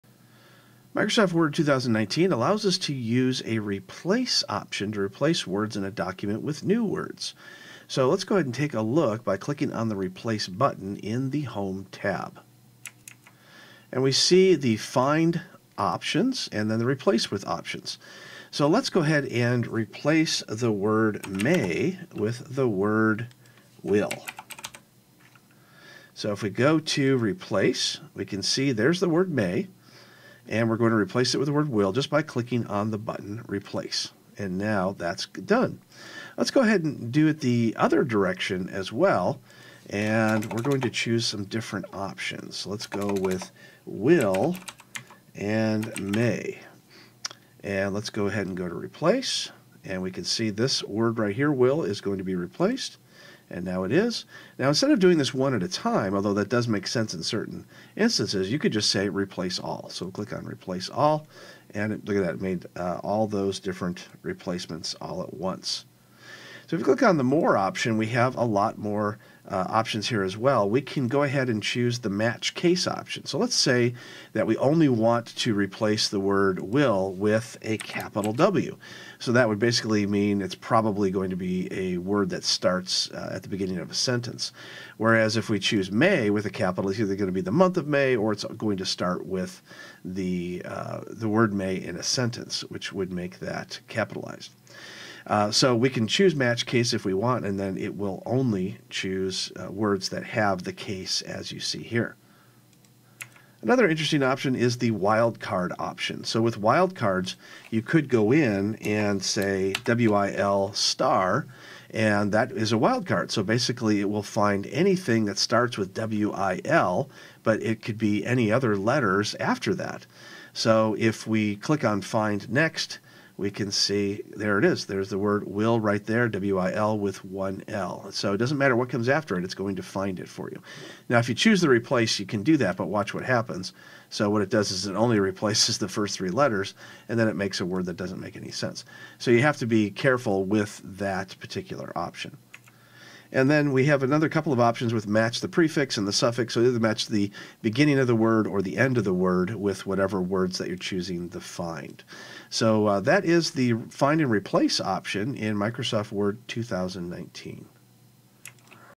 How to use find and replace in Word 2019
Professor Robert McMillen shows you how to use find and replace in Word 2019.